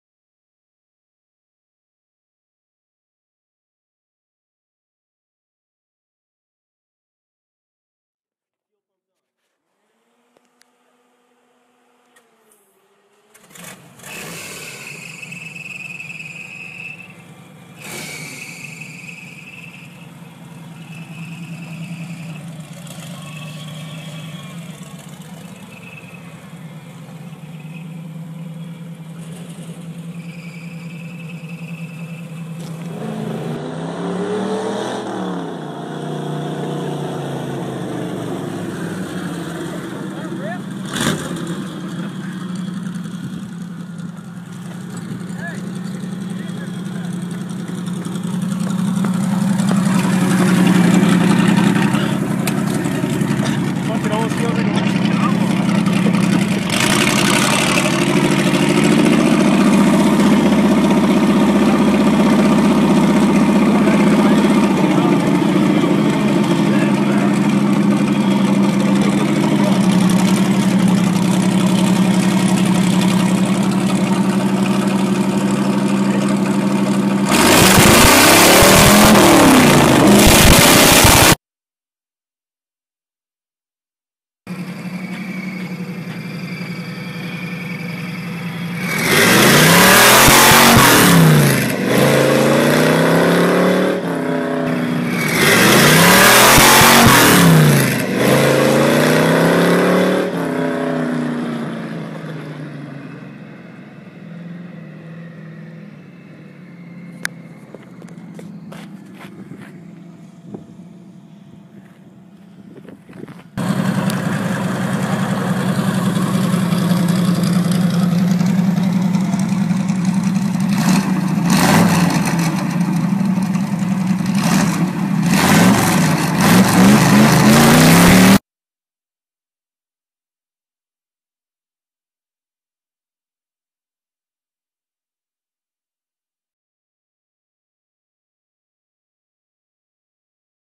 The Jeep Project
You guys saw the video last year when she rolled over on her side. Jesse put her back into the shop and has been tinkering with around here n there giving her a little make over.. Not done yet but today was her first day back outside rolling on all four! Here's a couple clips